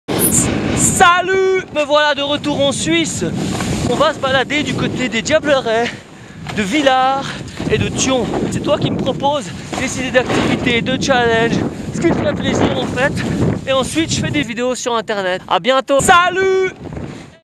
SUISSE MOI à la MAISON
Et voilà le prochaine trip ça sera SUISSE MOI à la MAISON du 13-19 février à Thyon, puis du 22-29 entre Villars et les Diablerêts.

Propose moi tes idées de défis, activités, personnes à rencontrer et tout ce qu'il te ferai plaisir à voir en vidéo. On diffusera une vidéo chaque 3-4 jours. Et attention une surprise vous sera proposée le dernier jour :) chuuuuut !!!